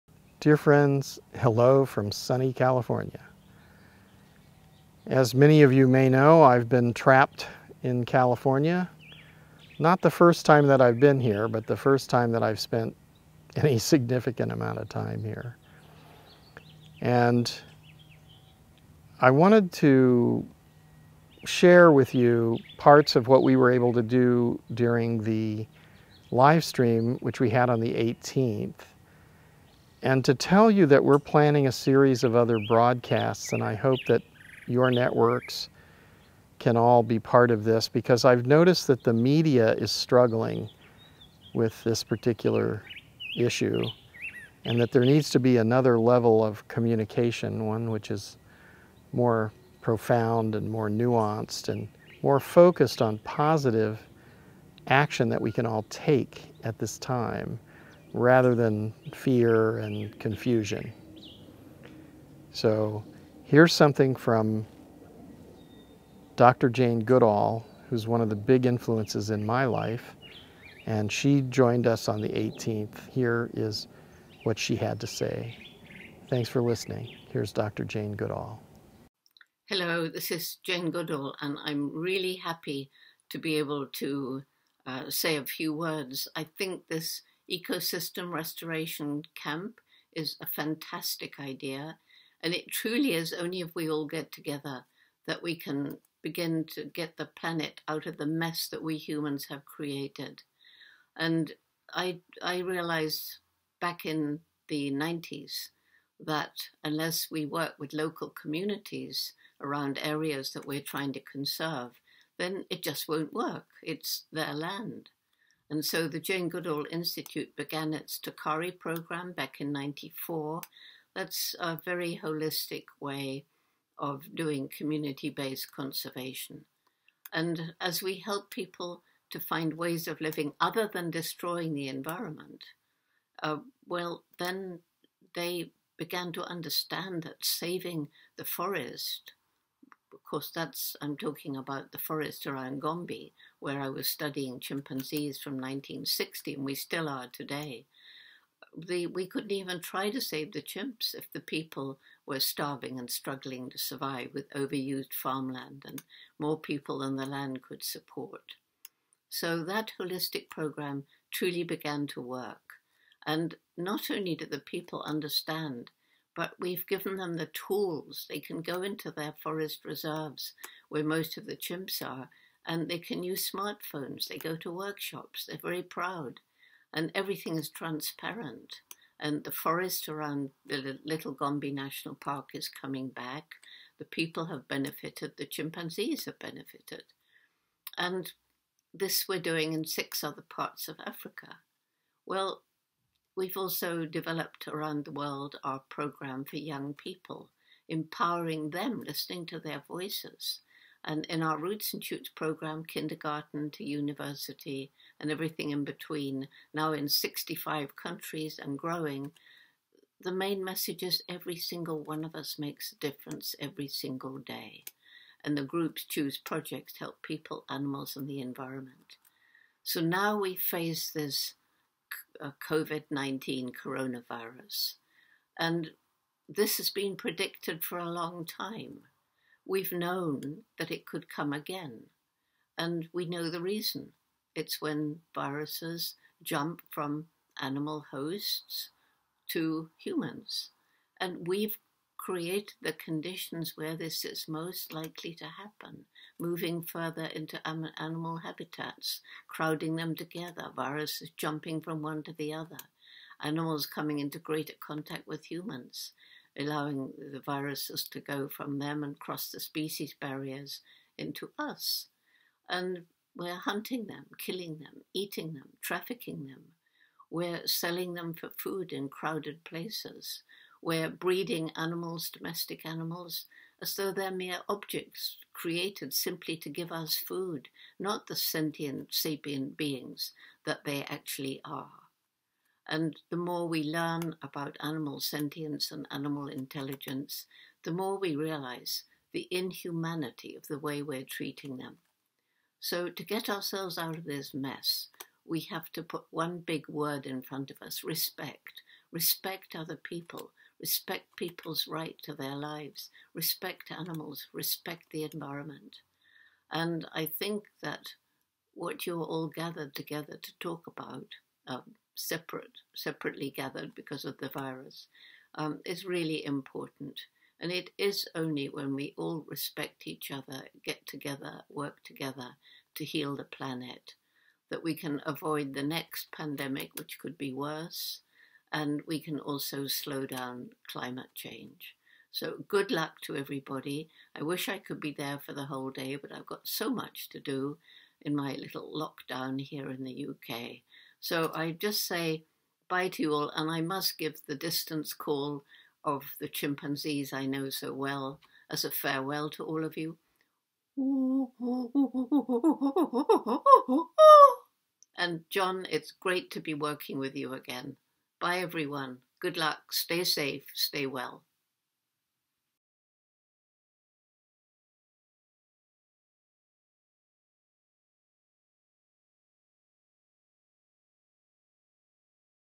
Jane Goodall
Dr. Jane Goodall joined "The Great Work of Our Time" Livestream from Woodshed Recording in Malibu, California. on the 18th of April. This is what Dr. Goodall had to say.